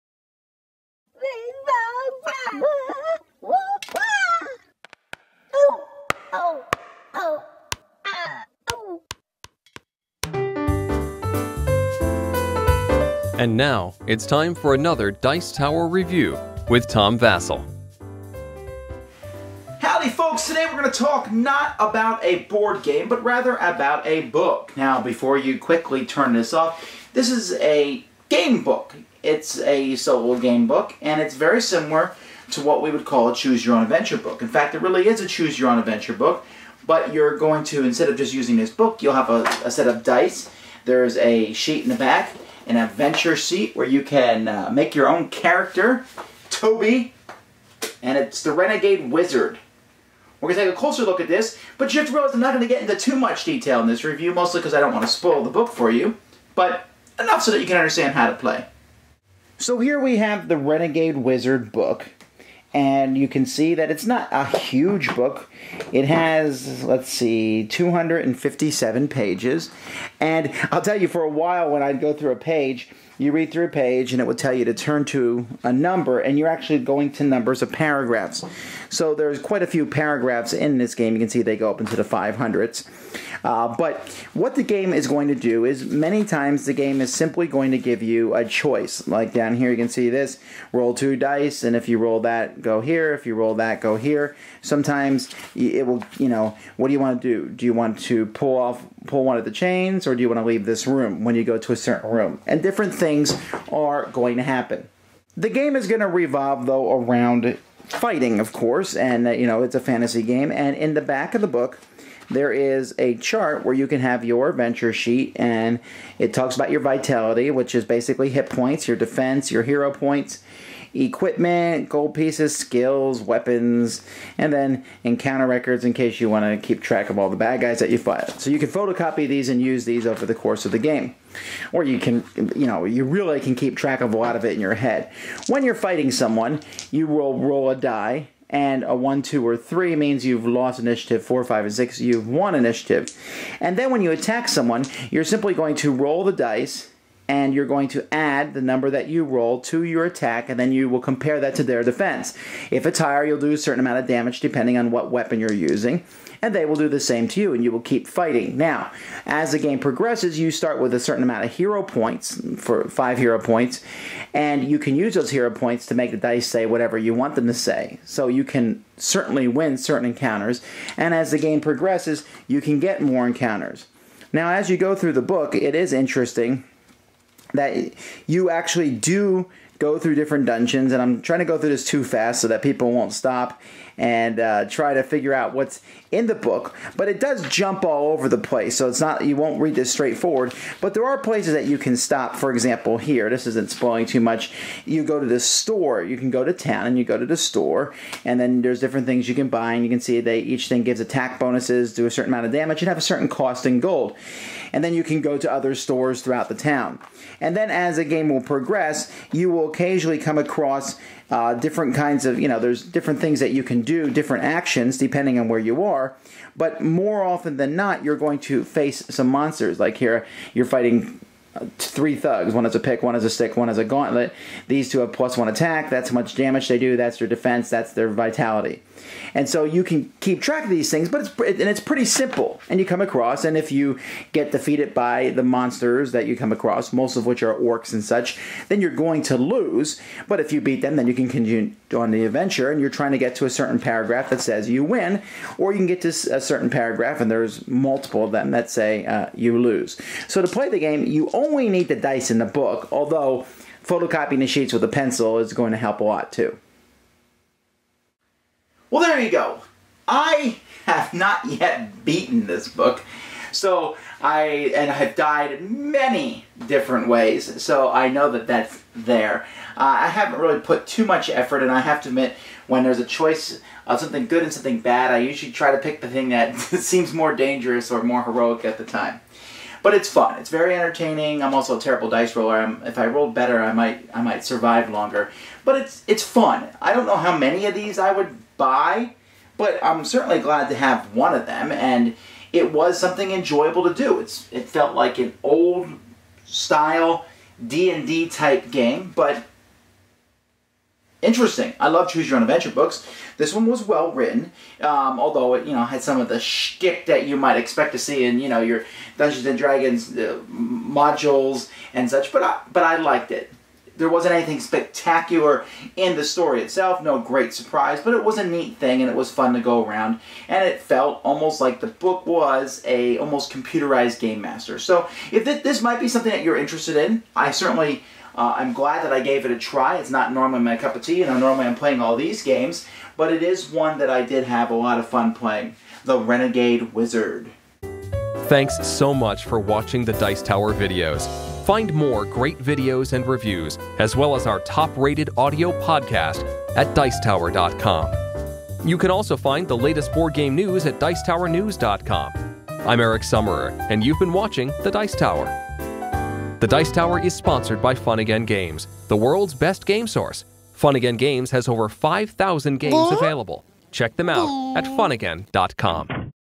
Renegade Wizard Review - with Tom Vasel
Tom examines this adventure book along the lines of "Choose Your Own Adventure"

00:00 - Introduction
00:59 - Overview
04:18 - Final Thoughts